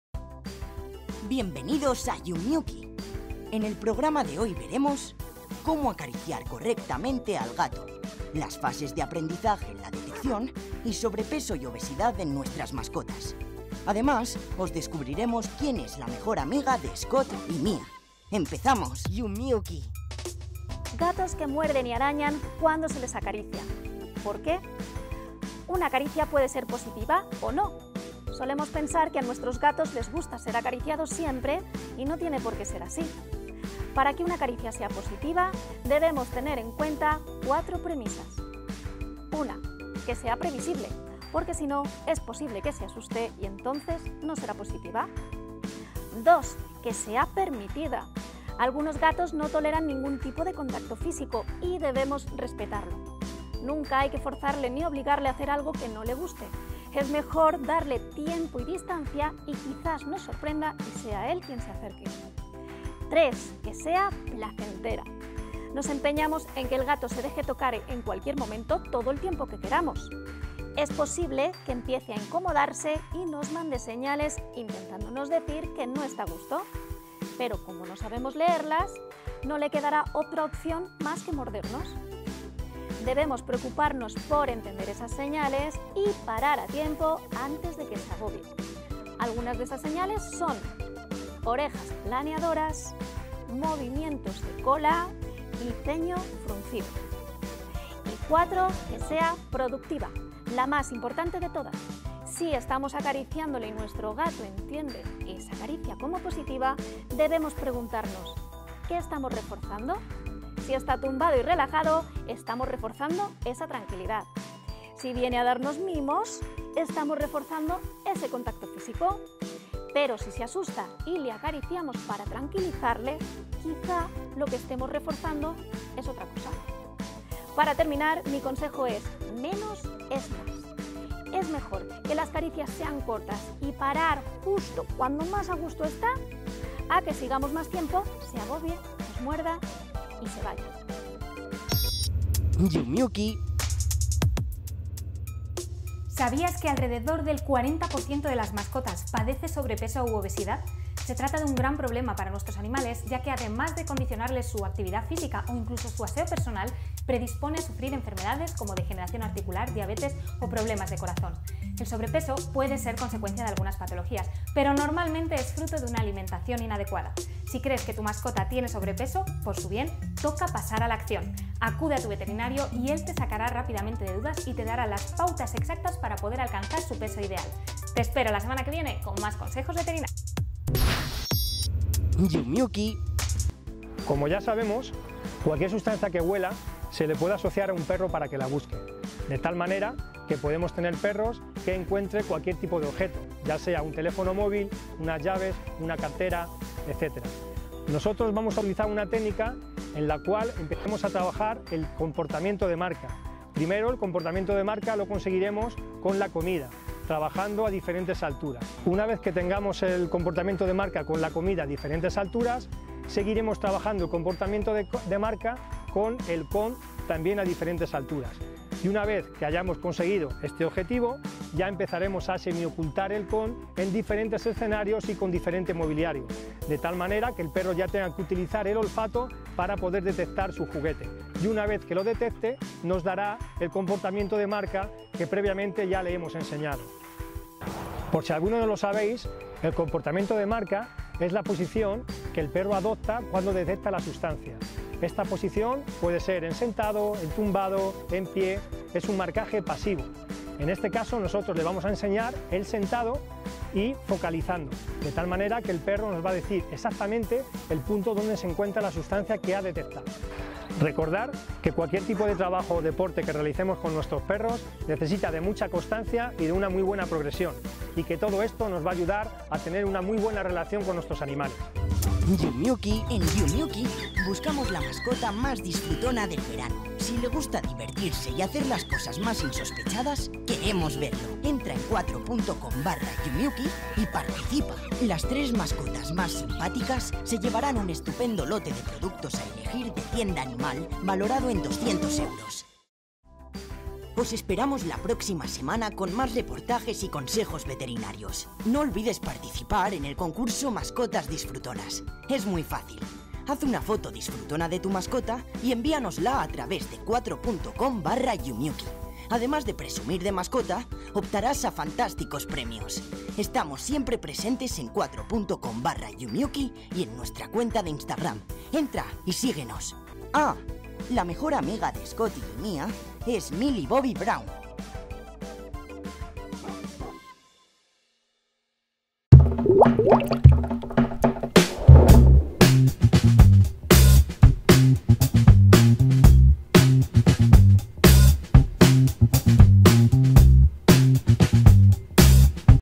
Cuidado con el sobrepeso en nuestros animales de compañía | Mediaset
Todos sabemos lo complicado que en ocasiones puede resultar acariciar a un gato; malas reacciones, mordidas o simplemente se apartan. Esto se debe a que a los gatos no siempre les gusta ser acariciados.  Para cambiar este comportamiento es necesario tacto y seguir los cuatro consejos que os daremos en el día de hoy.

¿Sabías que el cuarenta por ciento de las mascotas sufre sobrepeso? Esto es algo que por lo general se debe a una mala alimentación y a la vida sedentaria que siguen. No es difícil solucionar este problema pero es necesario detectarlo a tiempo. ¡Hoy te daremos los consejos veterinarios necesarios para mantener un buen nivel de salud en nuestras mascotas!

El experto en detección canina Alfredo Garrido nos hablará de la detección canina y nos recomendará ejercicios para fortalecer este rasgo olfativo de nuestros perros.